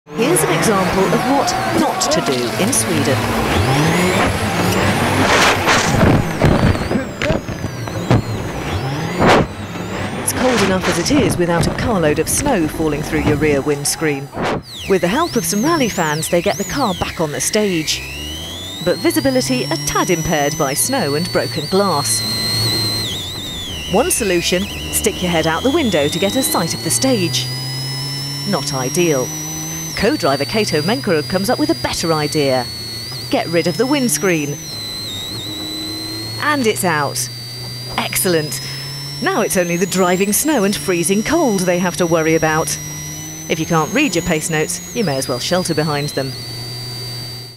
THAT Famous Solberg Snow Crash ❄️
One of the most memorable moments in WRC History...